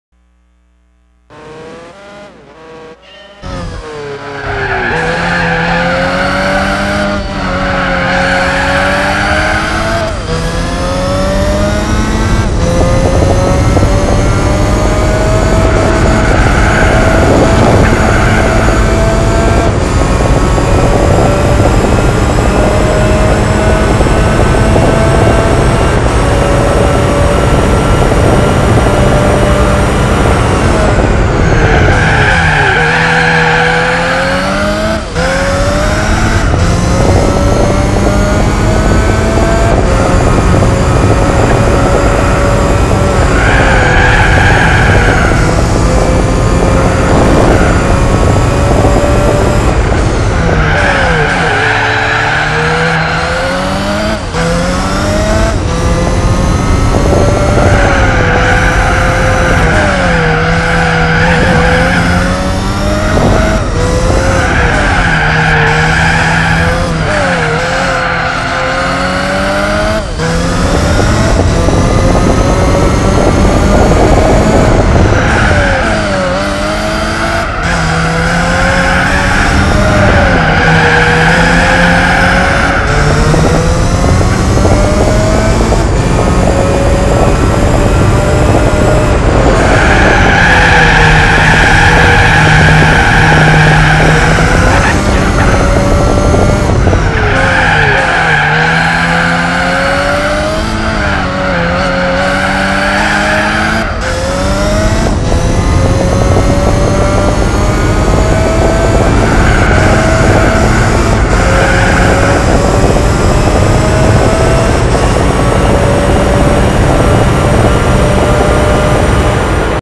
GTR Vspec II Nur: UNBEATABLE SUPER LAP! @ Grand Valley (2/2)
incar view